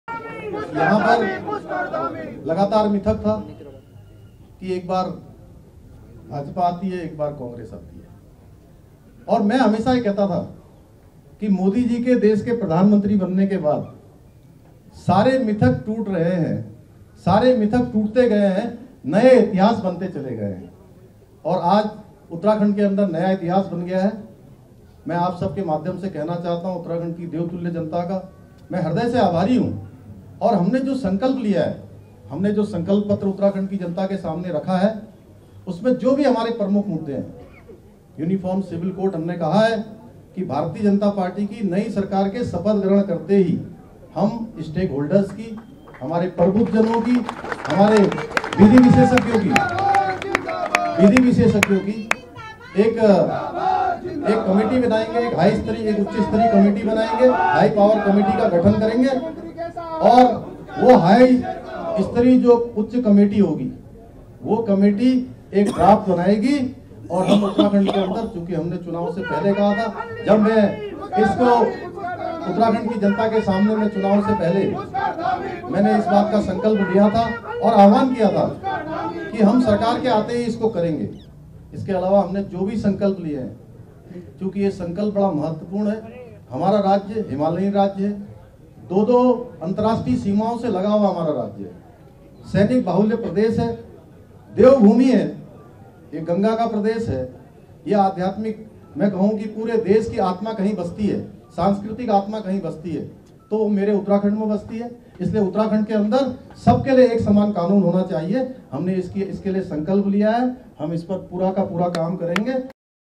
'मैं सीएम रहूं या नहीं, नई सरकार में जल्द लागू होगा यूनिफॉर्म सिविल कोड' : Pushakar Singh Dhami
Uttarakhand Assembly Elections में बीजेपी ने बहुमत के आंकड़े को पार कर लिया है. ऐसे में बीजेपी एक बार फिर उत्तराखंड में सरकार बनाने जा रही है. ऐसे में चुनावी नतीजों को देखते हुए बीजेपी प्रदेश प्रभारी प्रह्लाद जोशी मीडिया से मुखातिब हुए. जोशी ने कहा कि चुनाव से पहले बीजेपी ने जो भी वादे किये हैं, उन्हें पूरा किया जाएगा. वहीं, इस मौके सीएम धामी ने कहा कि प्रदेश में सरकार बनते ही सबसे पहले यूनिफॉर्म सिविल कोड लागू किया जाएगा.